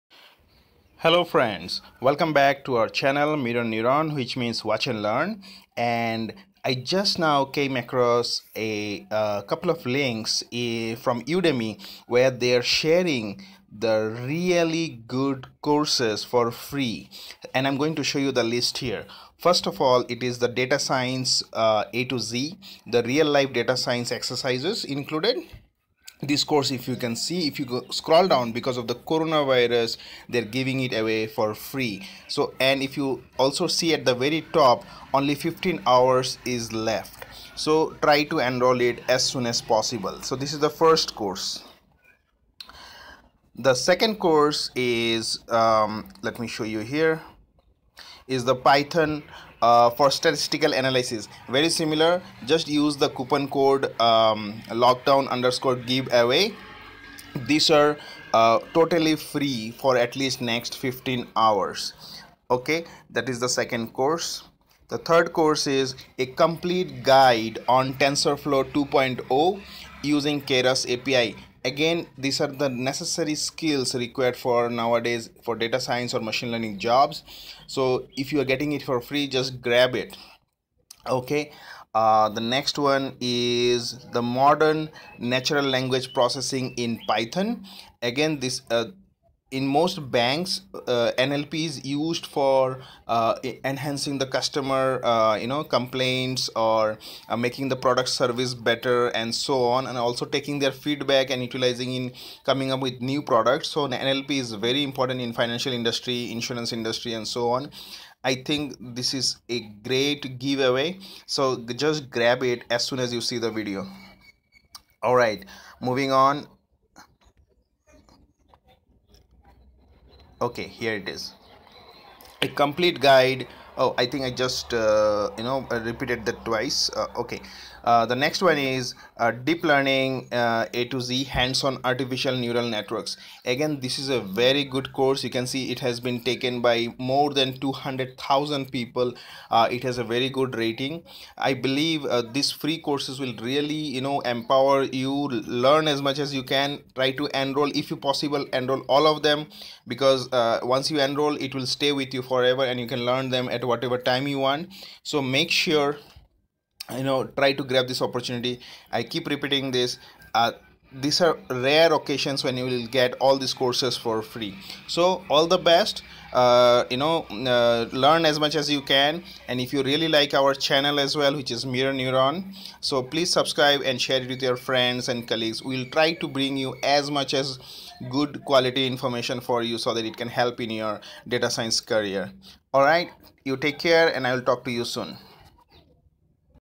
Top Udemy Courses for Free | Data Science | Python Programming
A. Basic Image Classification with Tensorflow
Solve classification problems with neural networks and Tensorflow.

B. Facial Expression Recognition with Keras
Deploy a trained CNN model to a web interface with Flask and Keras API. Apply the model to real-time video streams and image data.

C. Deploy Models with Tensorflow Serving and Flask
Create web application with Flask to work as an Interface to a served/trained model.

D. Understanding Deepfakes with Keras
Implement a Deep Convolutional Generative Adversarial Network (DCGAN). Synthesize realistic looking images.

E. Classify Radio Signals from Space using Keras
Analyse deep space radio signals from Allen Telescope Array, and image classification using CNN in Keras.

Top 20 Courses to learn while the market recovers from Coronavirus spread and finds treatment and cure.

N. Computational Thinking (must for DS leads)

100% Free courses